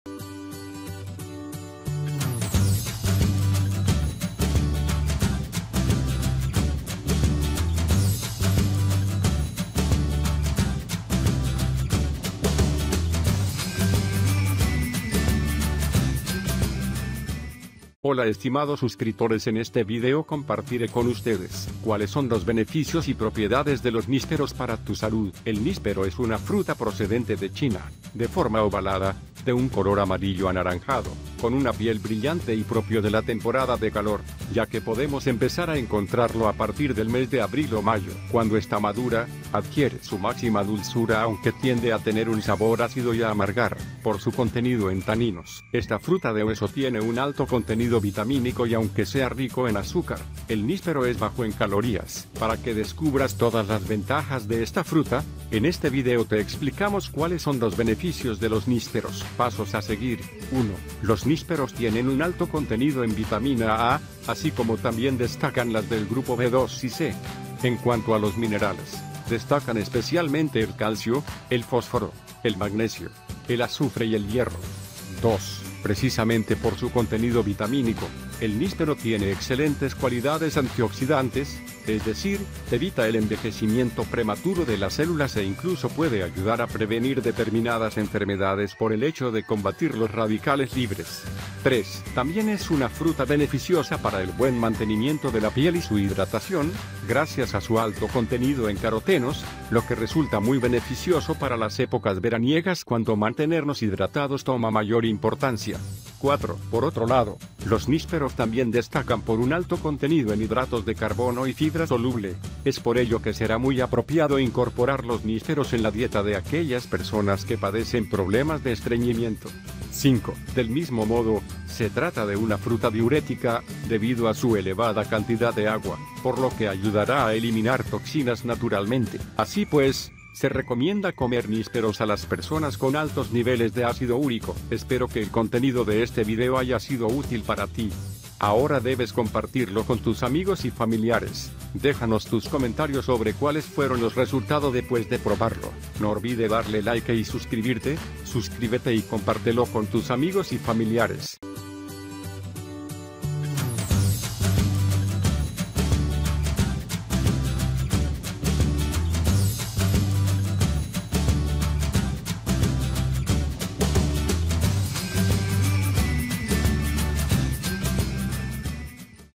Cuáles son los beneficios y propiedades de los nísperos para tu salud
El níspero es una fruta procedente de China, de forma ovalada, de un color amarillo anaranjado, con una piel brillante y propio de la temporada de calor, ya que podemos empezar a encontrarlo a partir del mes de abril o mayo. Cuando está madura, adquiere su máxima dulzura aunque tiende a tener un sabor ácido y a amargar, por su contenido en taninos. Esta fruta de hueso tiene un alto contenido vitamínico y aunque sea rico en azúcar, el níspero es bajo en calorías. Para que descubras todas las ventajas de esta fruta, en este video te explicamos cuáles son los beneficios de los nísperos.

Pasos a seguir:

1.
Los nísperos tienen un alto contenido en vitamina A, así como también destacan las del grupo B2 y C. En cuanto a los minerales, destacan especialmente el calcio, el fósforo, el magnesio, el azufre y el hierro.

2.
Precisamente por su contenido vitamínico, el níspero tiene excelentes cualidades antioxidantes, es decir, evita el envejecimiento prematuro de las células e incluso puede ayudar a prevenir determinadas enfermedades por el hecho de combatir los radicales libres.

3.
También es una fruta beneficiosa para el buen mantenimiento de la piel y su hidratación, gracias a su alto contenido en carotenos, lo que resulta muy beneficioso para las épocas veraniegas cuando mantenernos hidratados toma mayor importancia.

4.
Por otro lado, los nísperos también destacan por un alto contenido en hidratos de carbono y fibra soluble, es por ello que será muy apropiado incorporar los nísperos en la dieta de aquellas personas que padecen problemas de estreñimiento.

5.
Del mismo modo, se trata de una fruta diurética, debido a su elevada cantidad de agua, por lo que ayudará a eliminar toxinas naturalmente. Así pues, se recomienda comer nísperos a las personas con altos niveles deácido úrico.